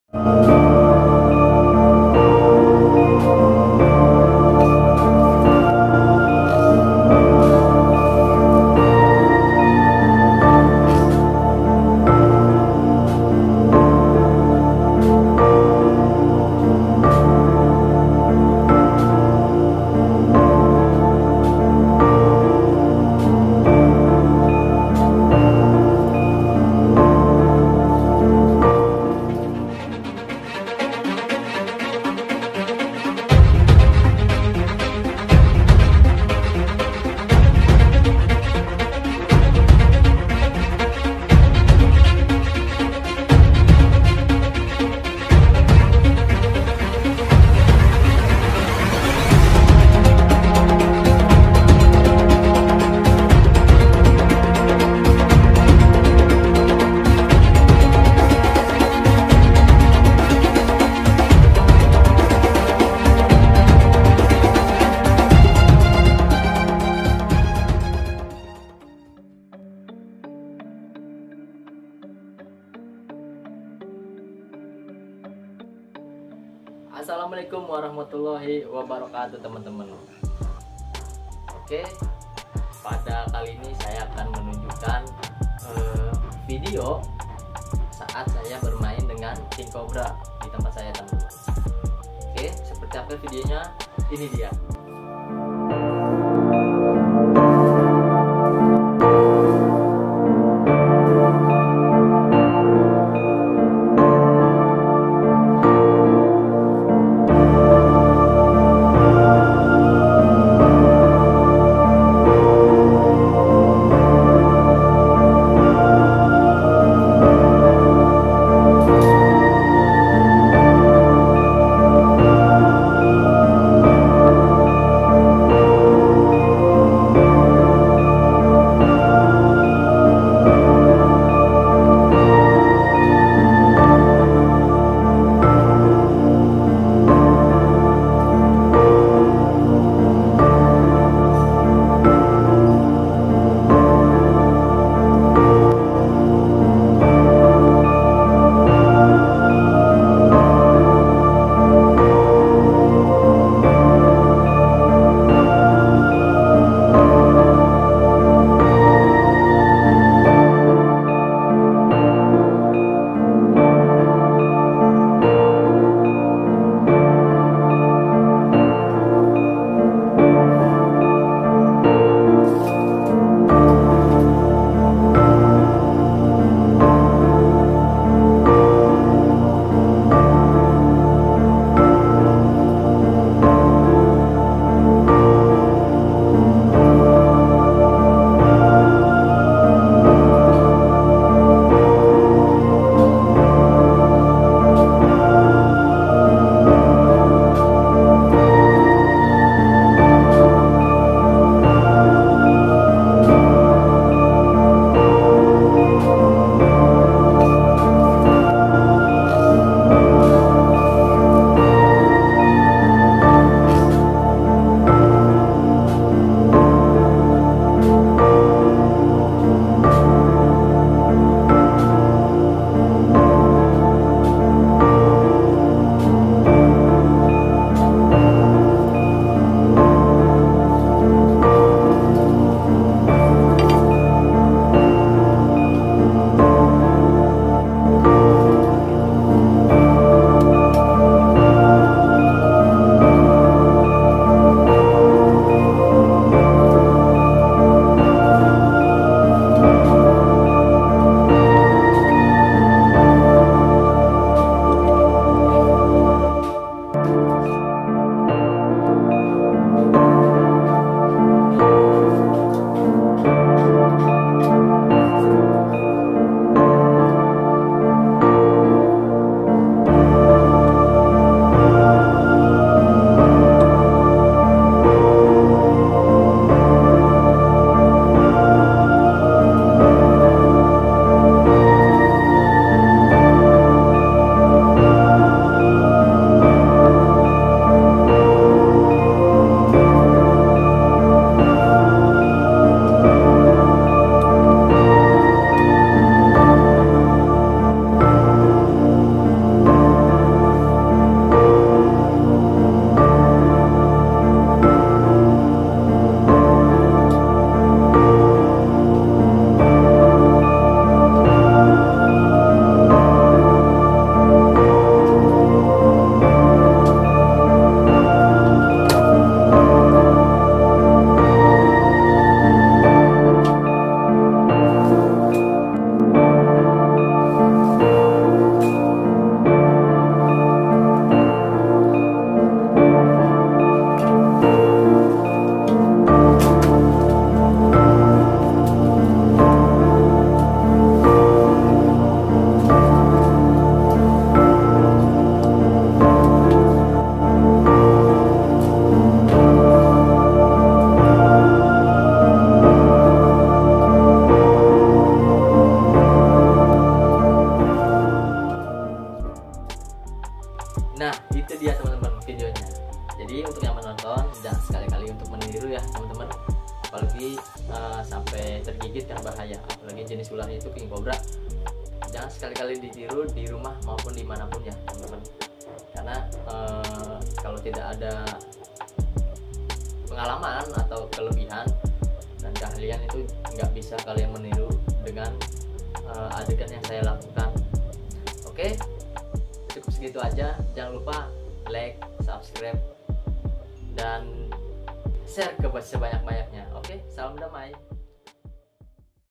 BERMAIN DENGAN ULAR KING COBRA || Ophiopaghus Hannah
Salam Damai @Kang Ari Channel
Oke Kali Ini Saya Akan Bermain Dengan Ular King Cobra. Untuk Yang Menonton Jangan Di Tiru Di Rumah Dan Dimana Pun Yah. Karna Kalo Tanpa Pengalaman Dan Keahlian Khusus. Kalian Tidak Bisa Melakukan Apa Yang Kami Lakukan,
Simak VideoNya Sampai Habis...
ENJOOYYY...

Terimakasih 😁🙏

PERHATIAN!
ADEGAN DALAM VIDEO INI SANGAT BERBAHAYA!
TIDAK UNTUK DITIRU DAN DILAKUKAN OLEH
SIAPAPUN DAN DIMANAPUN!